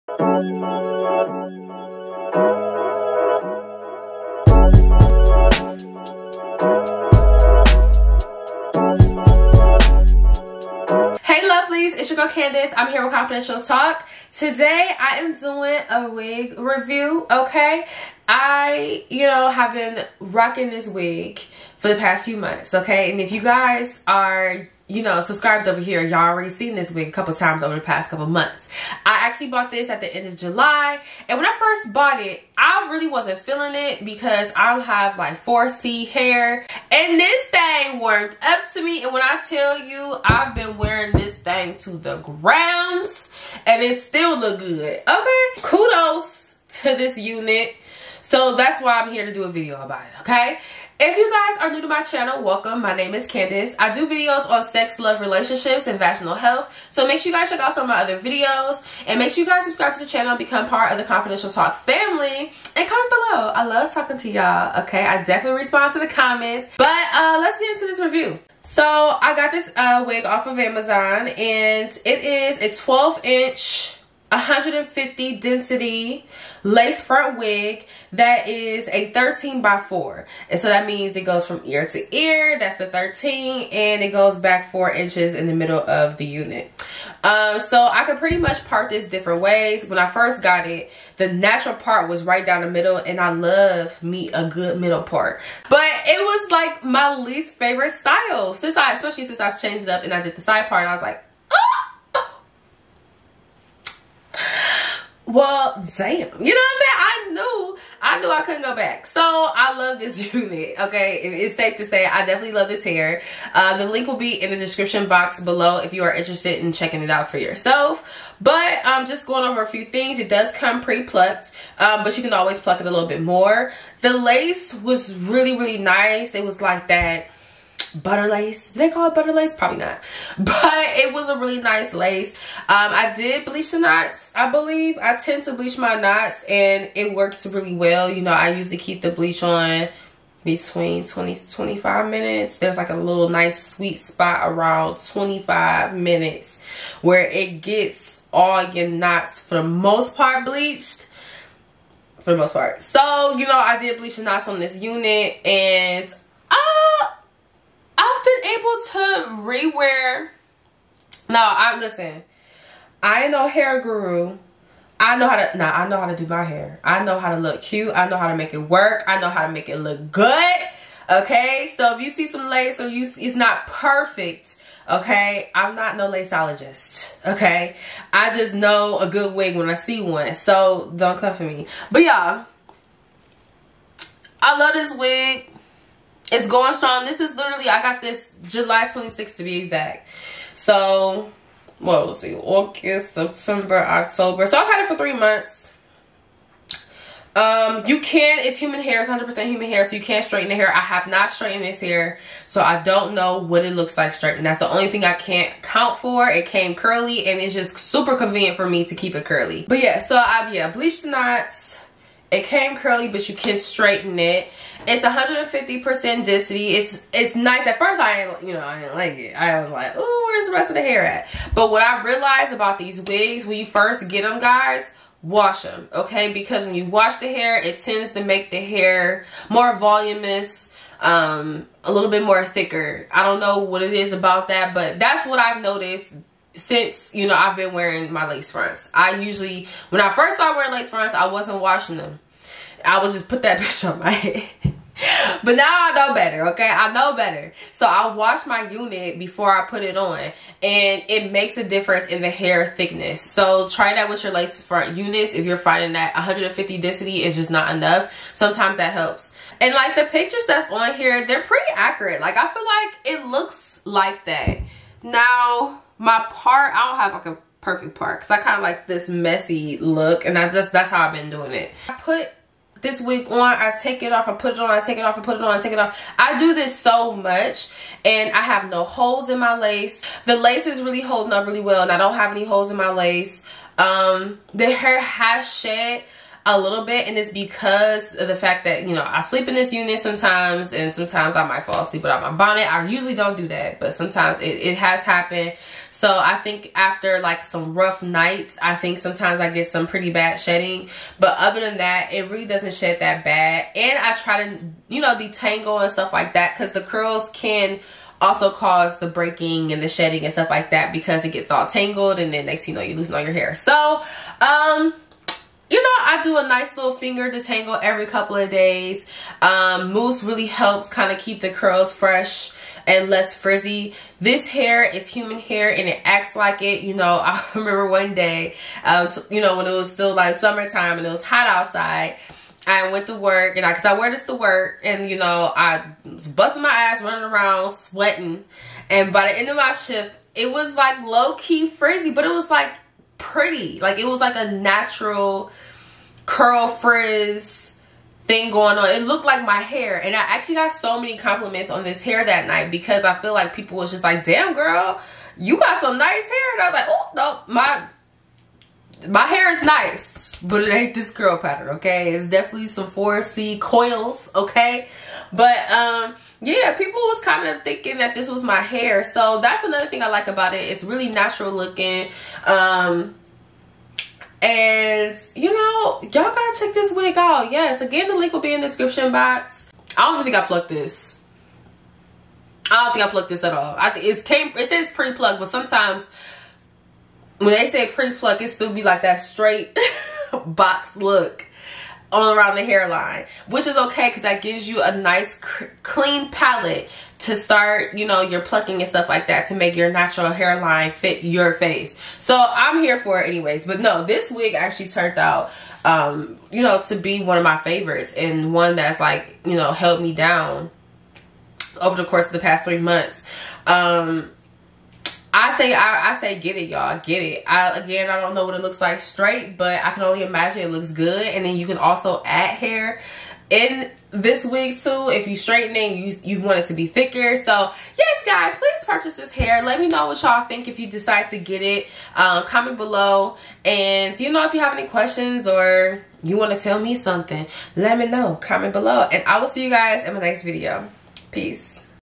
Amazon Wig Review! Short Curly 13x4 Lace Front Human Hair Wig (Detailed + Link)
And its part of prime!!! If yall not a PRIME member, yall are missing out!

Curly Wig

🌹Make sure you follow the Instagram to keep up with Confidential Talk Daily! @confidentialtalkbyglam